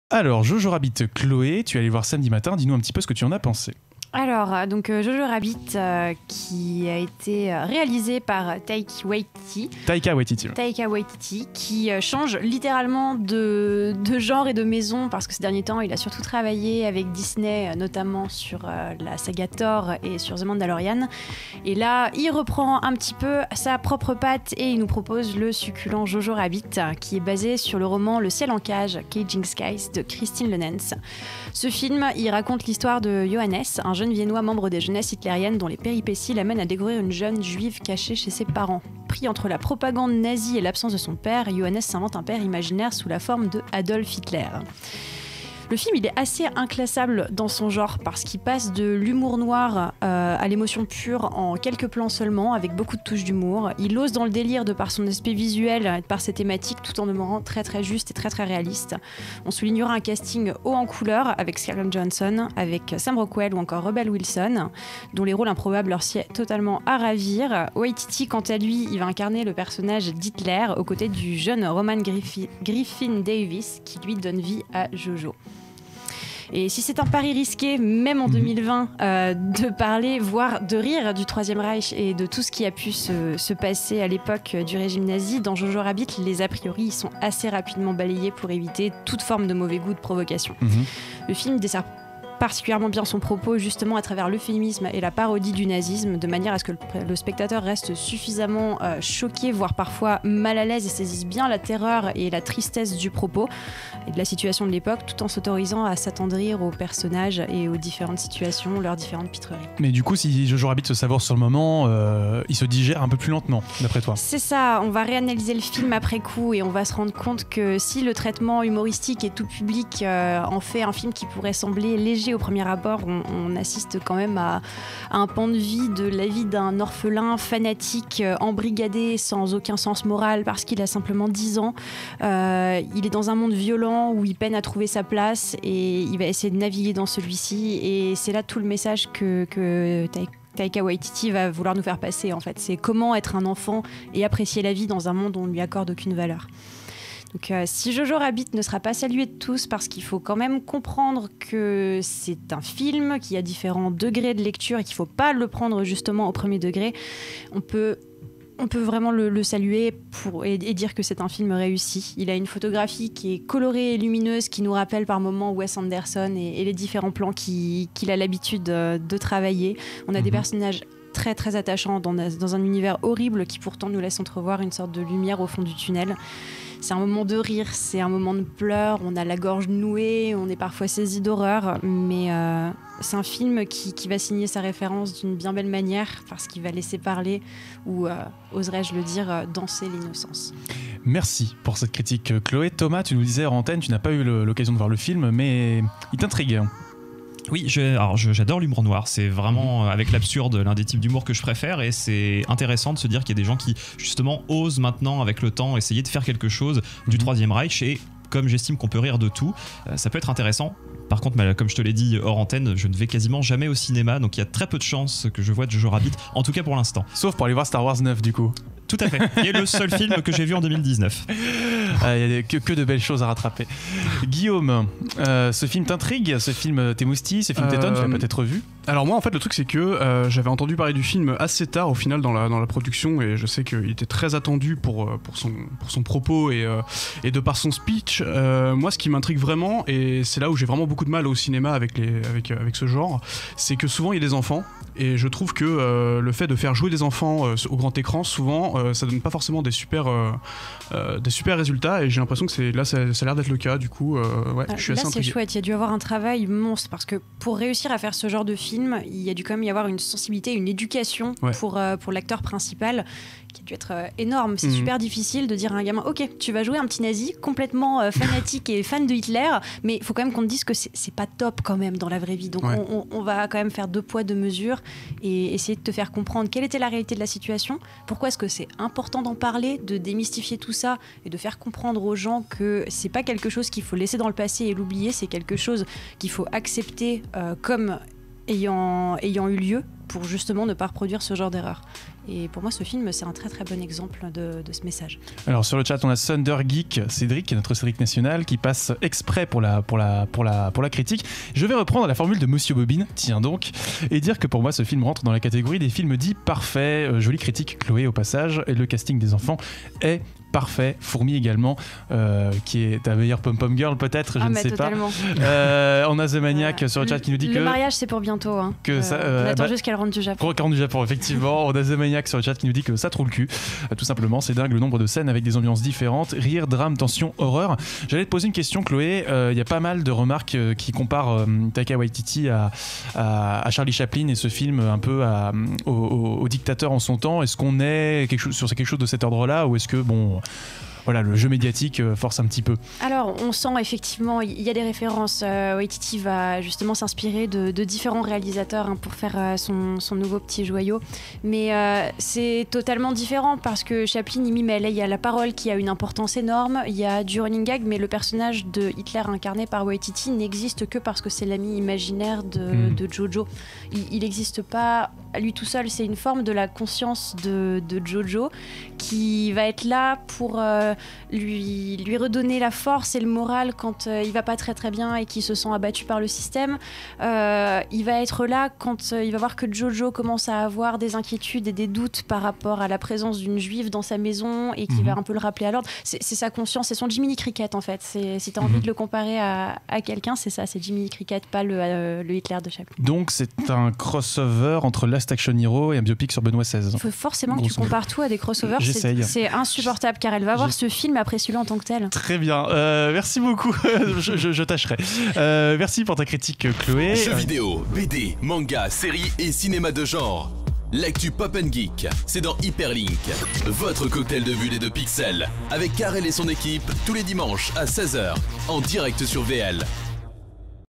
HyperLink #115 - Critique : Jojo Rabbit w/ Thomas PILLON
Allemagne, Seconde Guerre mondiale. Pour oublier les brimades dont il fait l’objet au sein des jeuness hitlériennes, le jeune Johannes “Jojo” Betzler se console avec son ami imaginaire et père de substitution : Adolf Hitler. Tout bascule le jour où Jojo découvre que sa mère cache une jeune fille juive dans son grenier… Plus proche d’À la poursuite de Ricky Baker que de Thor Ragnarok, ce nouveau film de Taika Waititi joue la carte de la comédie douce-amère dans un cadre risqué. Pari audacieux ou terrain miné ? À Chloé de trancher !